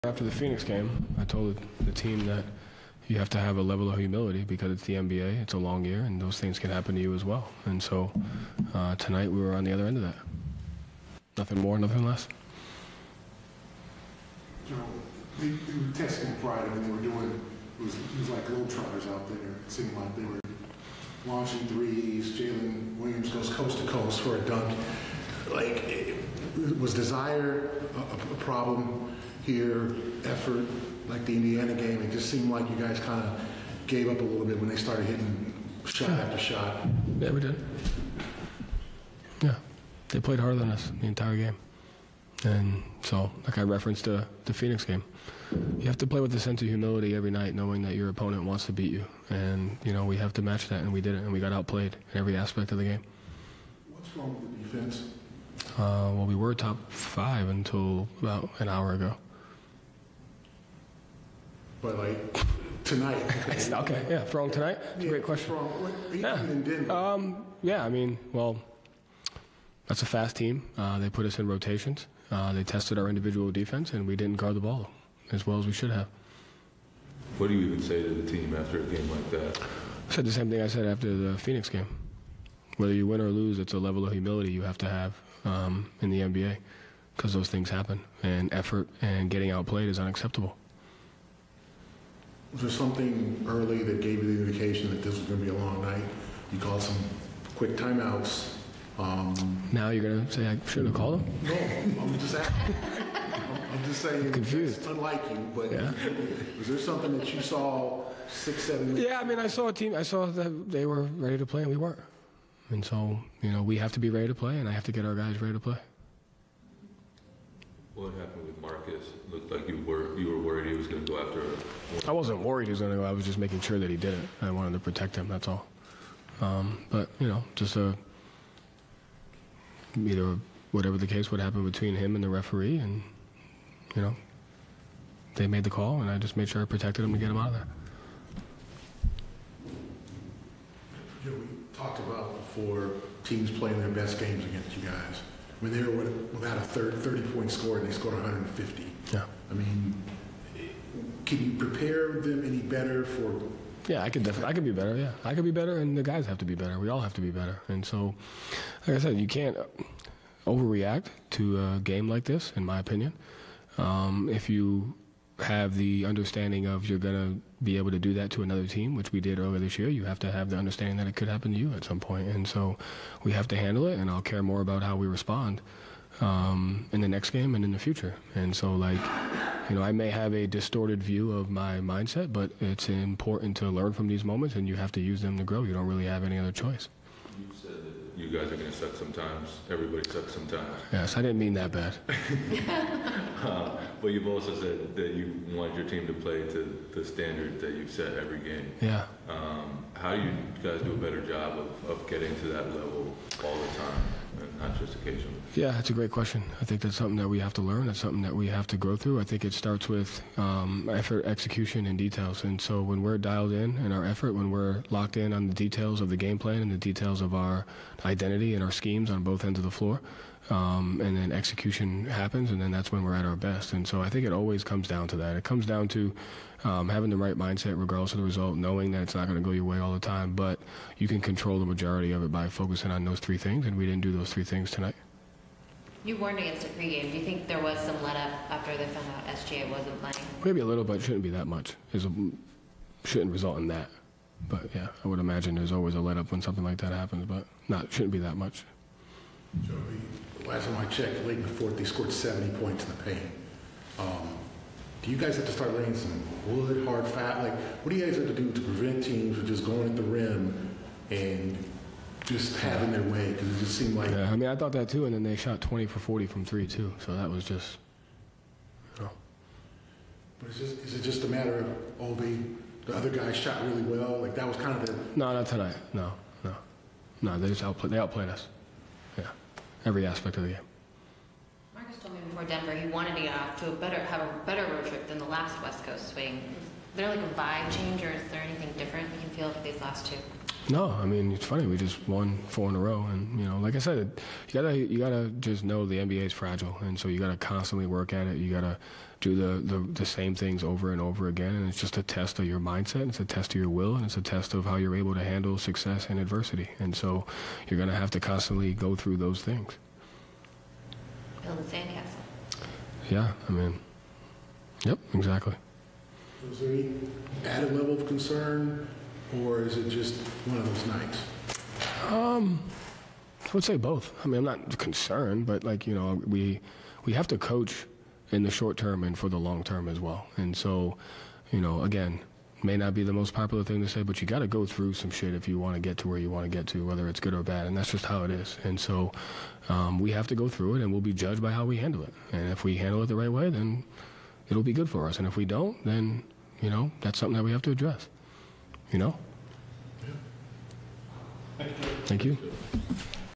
Joe Mazzulla postgame interview OKC 150, BOS 117
Boston Celtics head coach Joe Mazzulla postgame interview after loss vs Oklahoma City Thunder 150-117